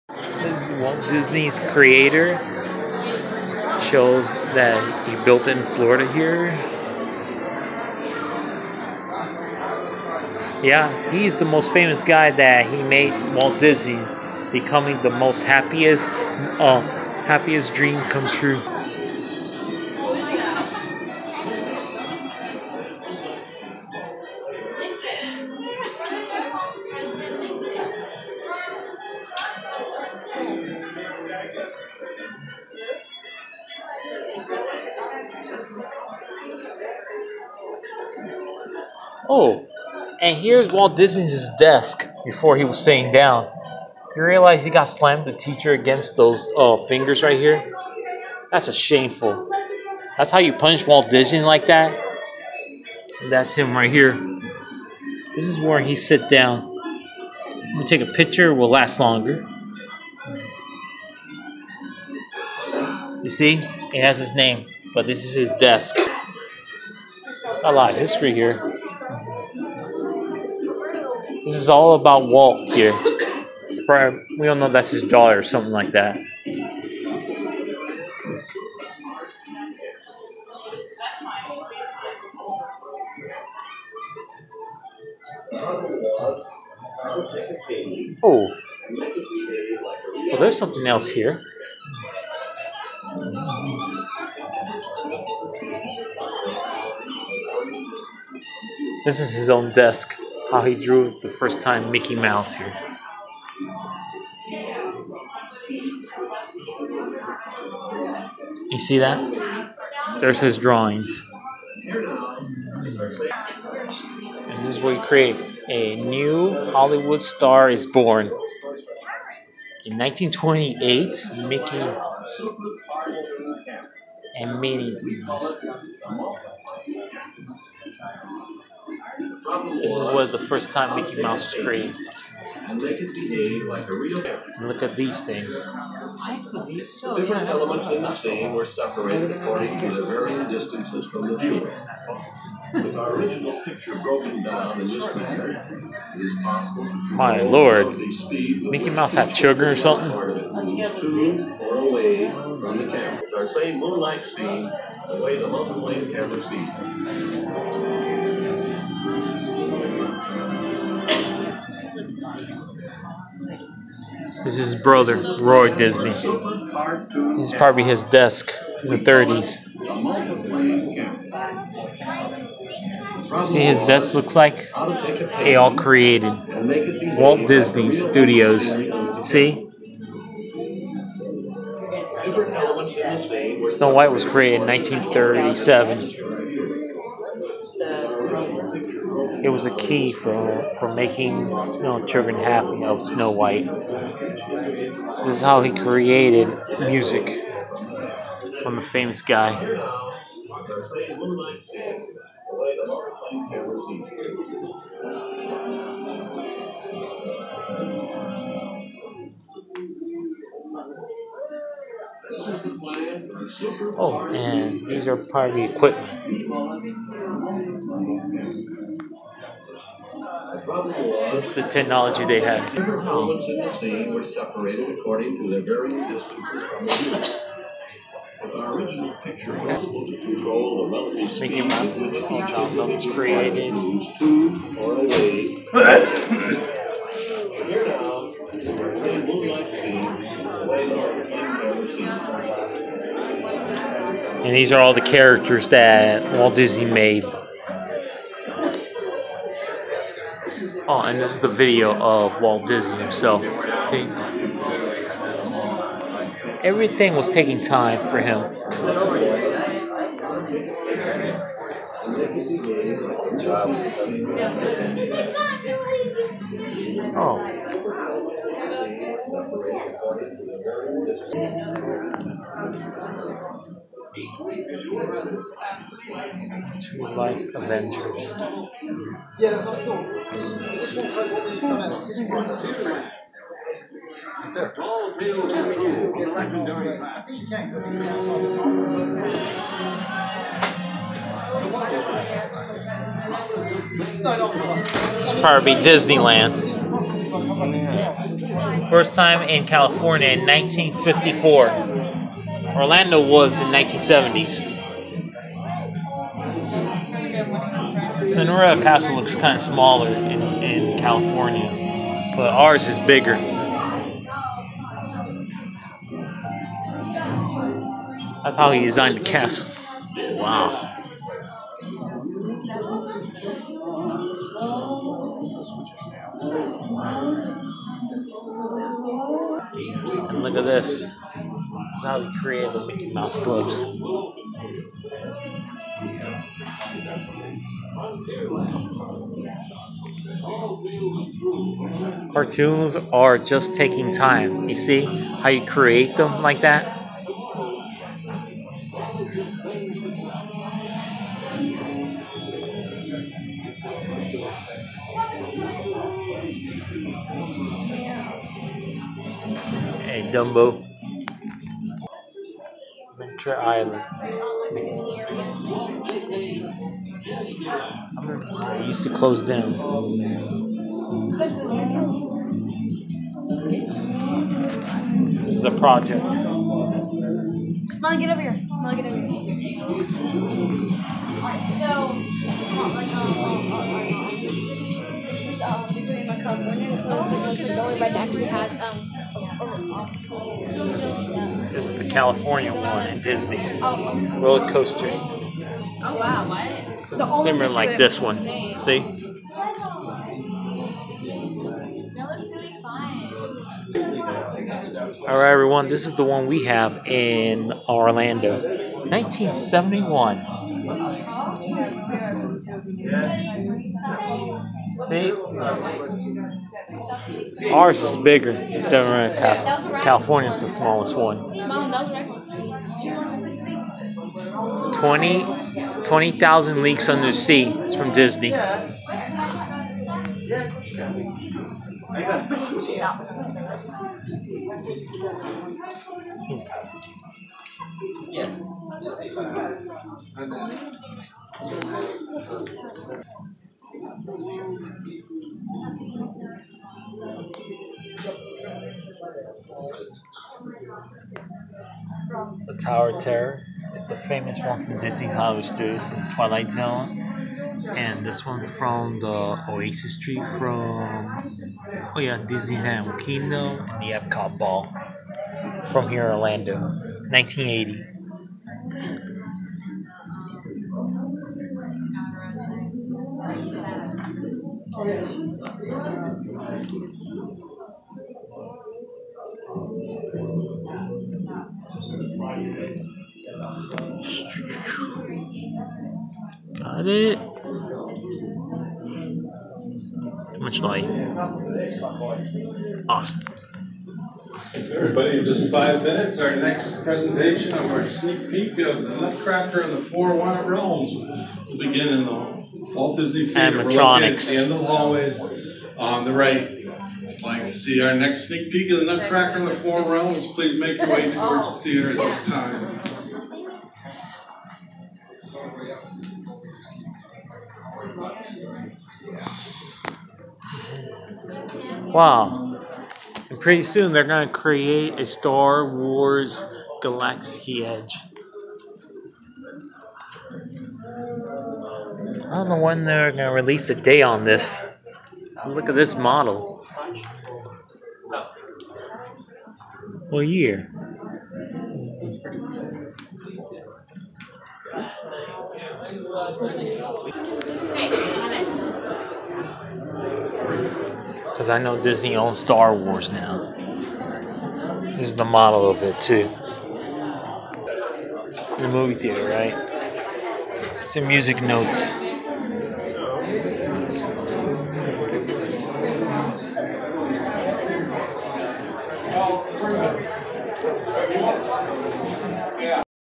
My Avengers in Disney Hollywood Studios 2018 part(10)
Never been to Disney's Hollywood Studios this is our first time and the same thing that my family never been Disney's Hollywood Studios I was pretty awesome.